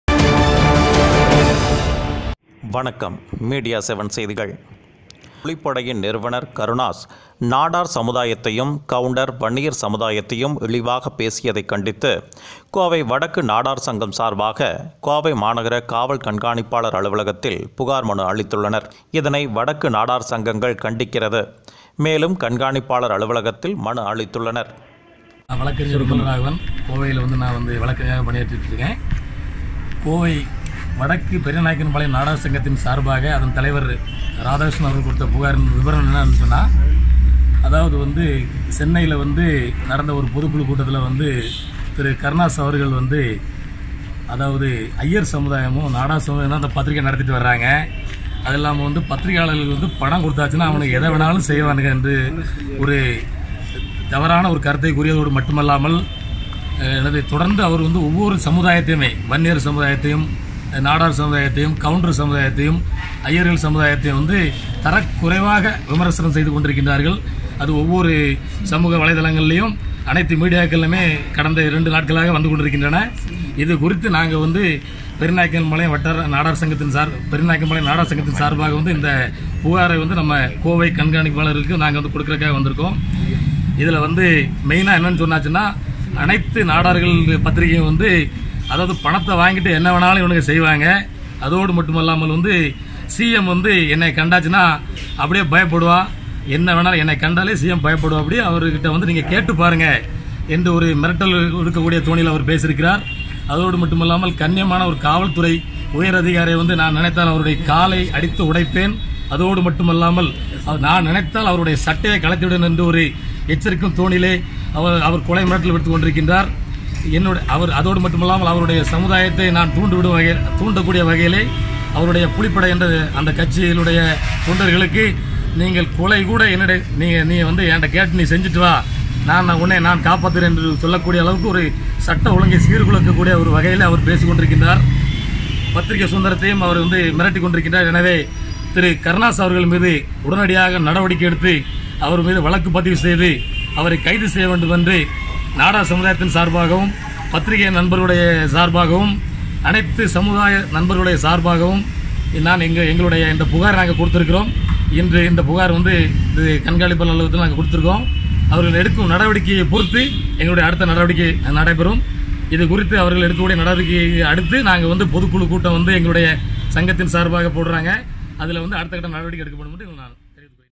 நடிகா் கருணாஸ் நாடார், கவுண்டர், வன்னியர், சமுதாயத்தை இழிவாகப் பேசியதைக் கண்டித்து
நடிகா் கருணாஸ்  நாடார், கவுண்டர், வன்னியர், சமுதாயத்தை இழிவாகப் பேசியதைக் கண்டித்து கோவை  நாடார் சங்கம் சார்பாக கோவை  காவல் கண்காணிப்பாளர்  அவா்களிடம்  புகார் மனு  அளித்தாா்கள்